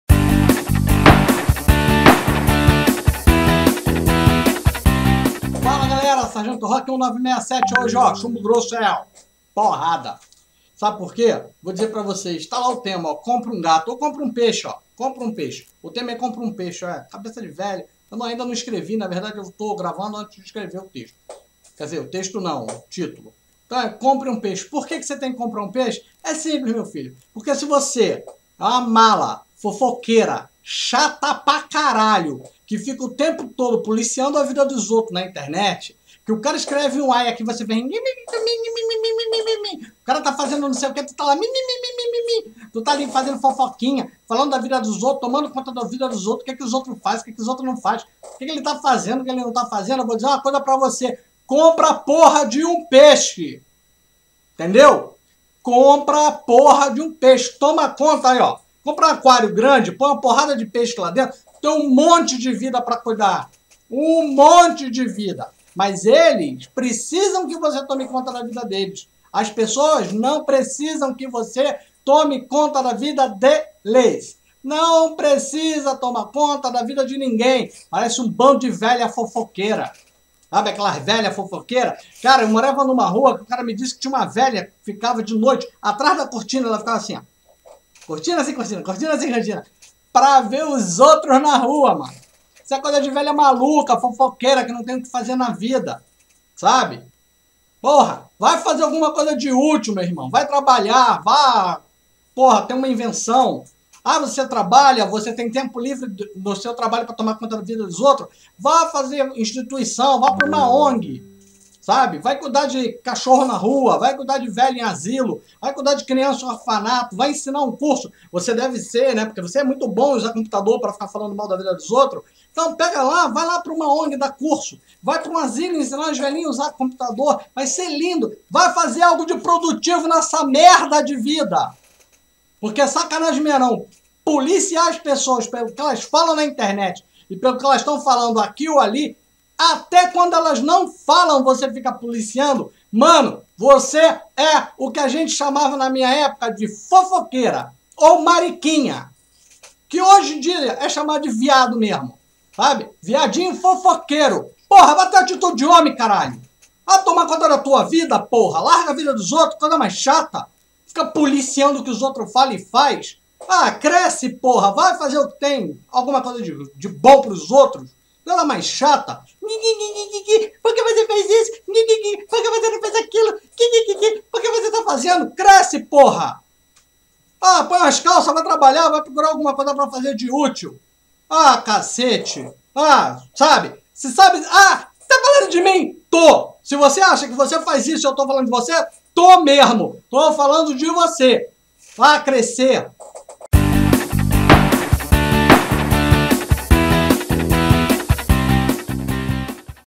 Chumbo Grosso – Compre um Peixe – Nós Nerds
Coluna de opinião com o fundador do Nós Nerds. Hoje o tema é Compre um peixe e vá tomar conta da vida deles!